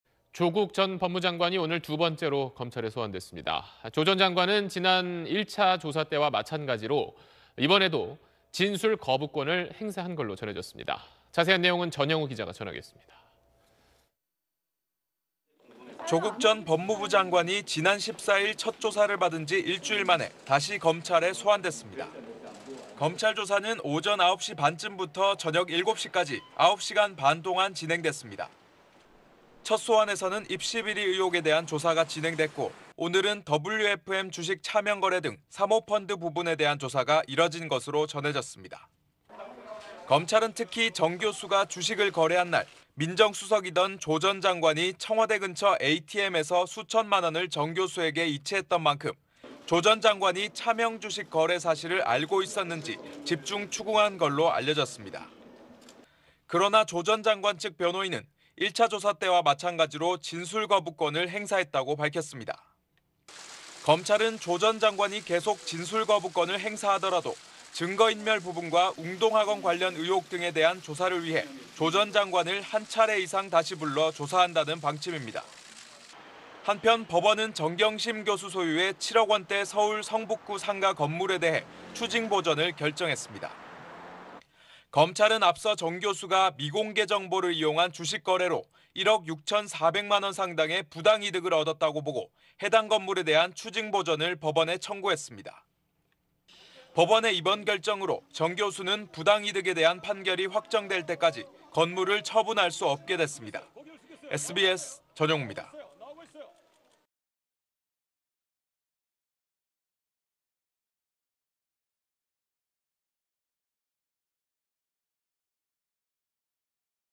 조국, 2차 소환도 진술 거부…부인 상가는 '추징보전' / SBS
조국 전 법무장관이 오늘(21일) 두 번째로 검찰에 소환됐습니다. 조 전 장관은 지난 1차 조사 때와 마찬가지로 이번에도 진술거부권을 행사한 것으로 전해졌습니다.
자세한 내용은 전형우 기자가 전하겠습니다.